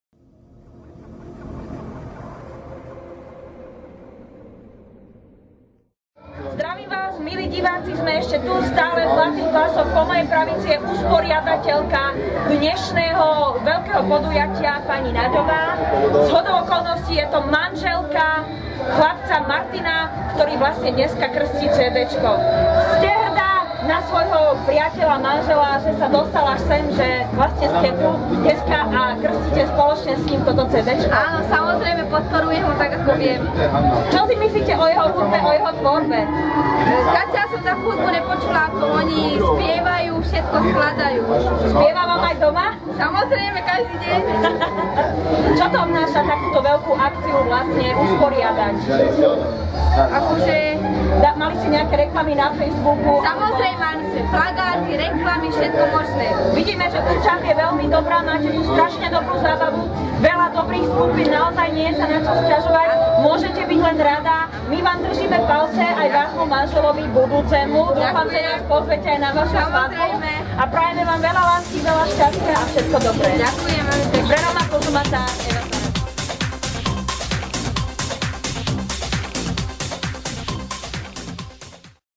Interview s Weronikou Nagyovou
Sympatická, nádherná už skoro pani WERONIKA NAGYOVÁ organizátorka skvelého podujatia Valentínska Zábava v Zlatých Klasoch, ktorá sa konala dňa 13. II. 2015. Na tomto výborne zorganizovanom podujatí vystúpili na počesť krstu CD skupiny MAXI BAND vynikajúci umelci ako je kráľ Rómskych piesní Kamil Polakovič, Tomáš Botló, Grezgeň - Mário a samozrejme MAXI BAND